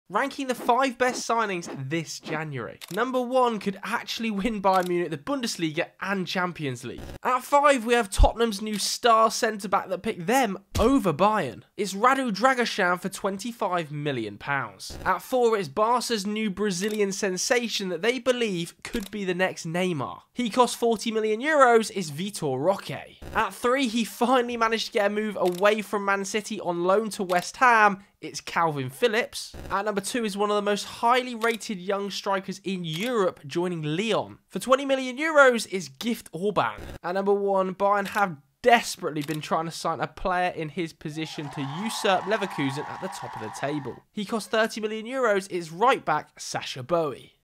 Ranking The 5 BEST Signings This January! ✍️💰
January might have been a quiet window this year, but there have still been some excellent signings. Including: Sacha Boey to Bayern Munich, Gift Orban to Lyon, Kalvin Phillips to West Ham, Dragusin to Spurs and Vitor Roque to Barcelona!